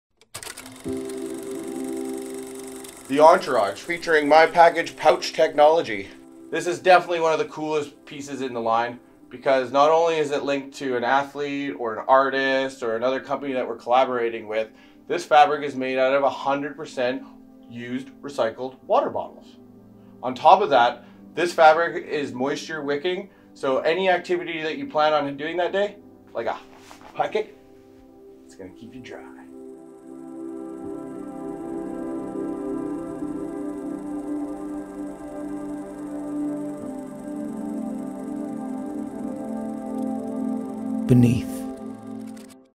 BN3TH Product Knowledge: Entourage
High-wicking for sweat. 100% post-consumer recycled polyester for Mother Earth.

These active Boxer Briefs are made from 100% post-consumer recycled plastic bottles and are inspired by our Entourage - a talented group of skaters, snowboarders, artists, surfers, bikers and all-around rad dudes. Our new EcoDry fabric is more durable and breathable than ever and features a thicker logo'd waistband for ultimate control.

TECHNICAL DETAILS
Slim fit with no fly
6.5 inch inseam
MyPakage Pouch Technology™
Seamless Pucker Panel
Thicker 1 3/4-inch luxury knitted waistband
86% Recycled Polyester / 14% Spandex
170 GSM fabric weight